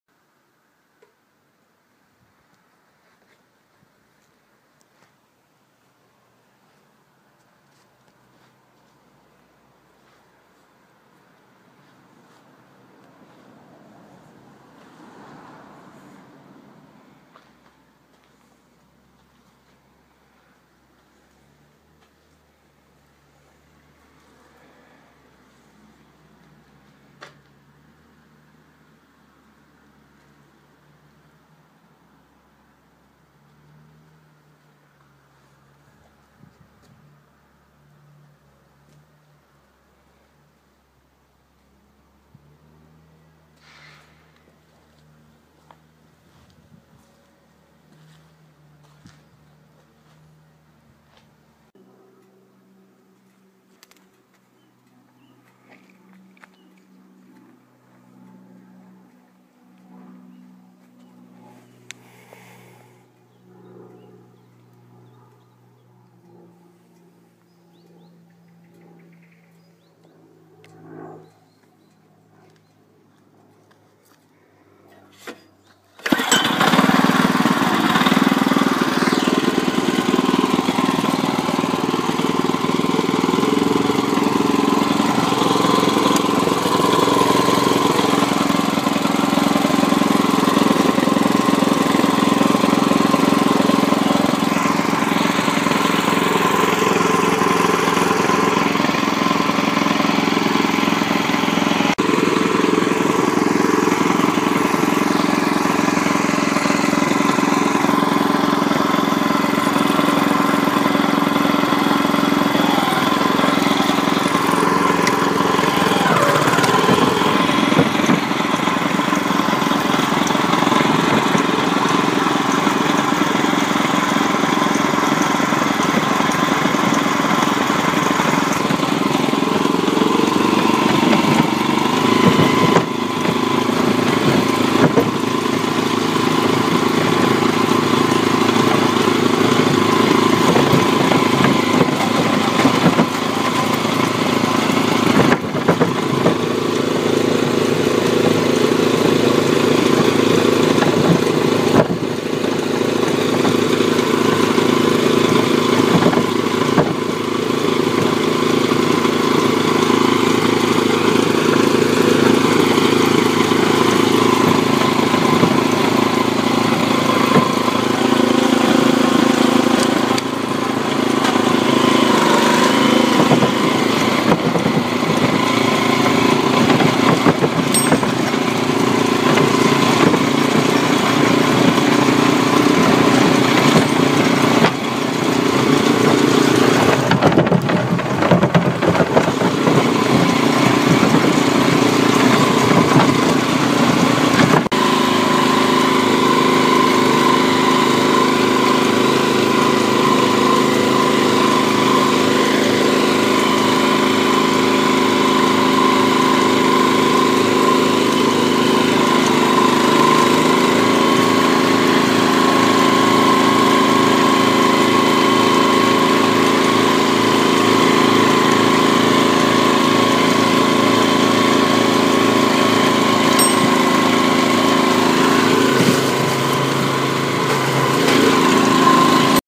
Kubota TF 65 | Einachsschlepper mit Anhänger Eigenbau | Einachser | motocultor 🚜
Moin,
hier gibt es eine kurze Vorstellung der Kubota TF 65. Der Anhänger ist selbstgebaut und lässt sich abbremsen. Zusätzlich ist eine Feststellbremse vorhanden.

Kubota TF 65 | Einachsschlepper mit Anhänger Eigenbau | Einachser | Kubota Kleintraktor | Kubota Traktor